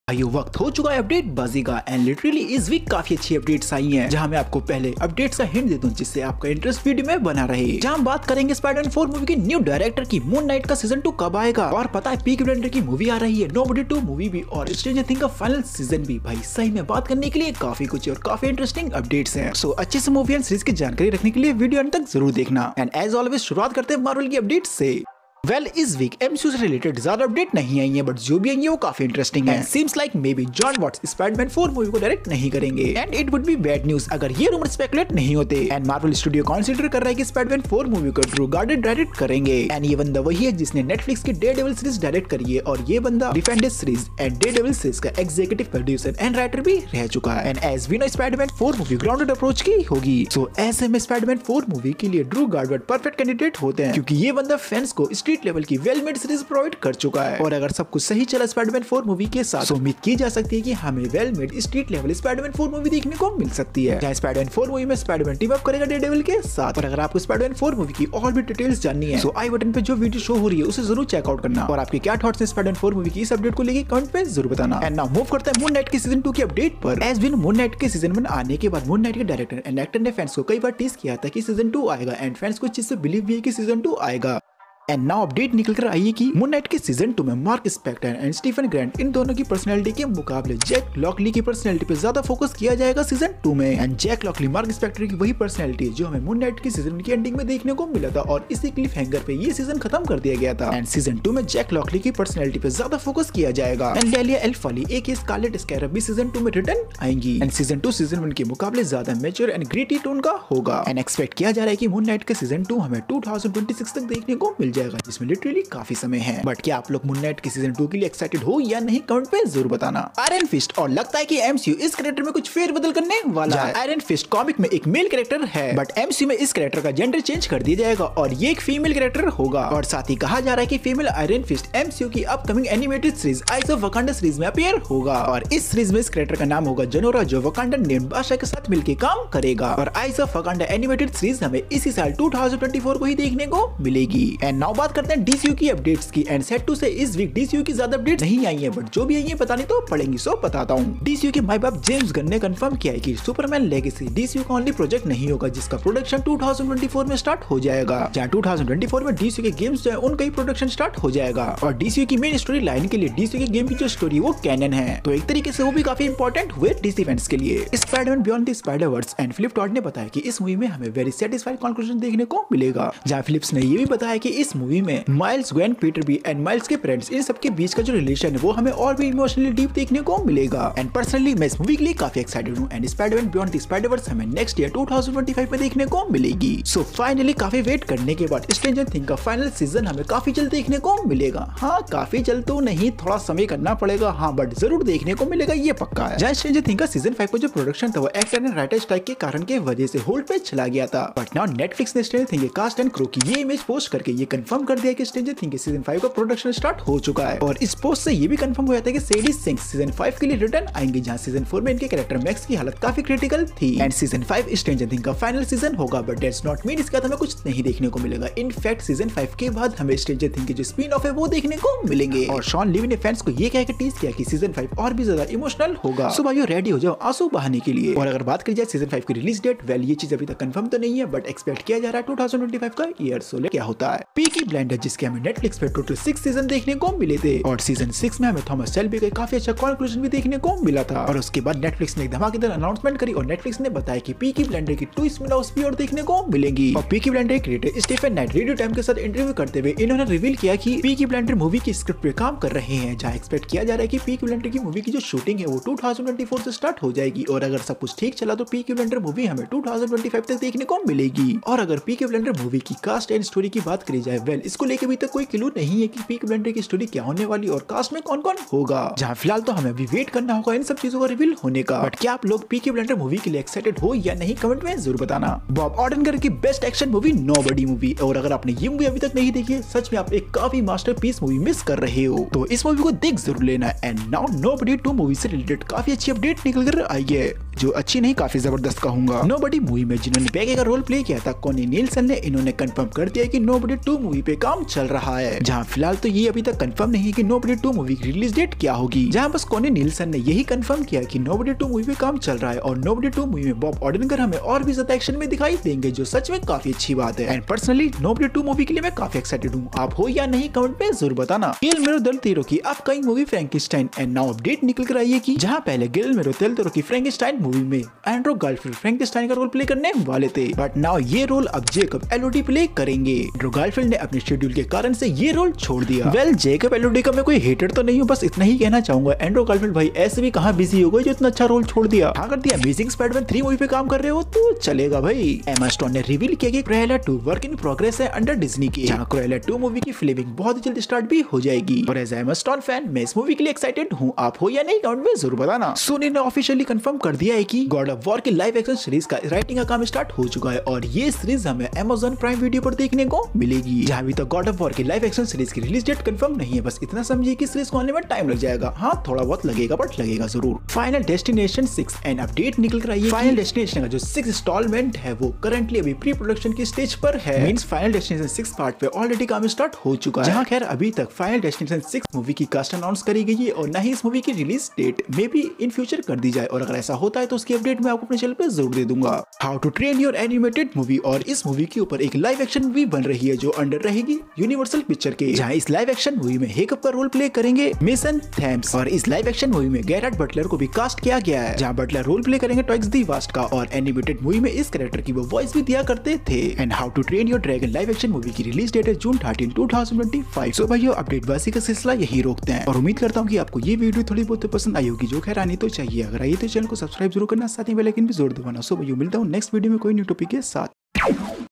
Moon Knight S-2 Confirmed, Peaky Blinders Movie, God of Wars Series, Stranger Things S-5, Ep-09
Peaky Blinders Ki Movie , Moon Knight S-2, Stranger Things S-5, UpdateBazzi Ep-09

TIMESTAMPS

00:00 Update-Bazzi Ep-09
00:25 Tom Holland Spider-Man 4 New Director
01:15 Moon Knight S-2 Update
02:00 Iron Fist in MCU
02:25 DCU update, Superman Legacy..
02:47 Spiderman Beyond The Spider Verse
03:06 Stranger Things S-5 Release Date
03:54 Peaky Blinders Movie Update
04:35 Nobody 2 Movie
05:11 Andrew Garfield Left Guillermo Del Toro Frankenstein
05:35 Emma Stone Cruella 2
05:45 God War Live Action Series
06:00 Final Destination 6 Update
06:19 How To Train Your Dragon Live Action Movie
06:41 The End.

Background Music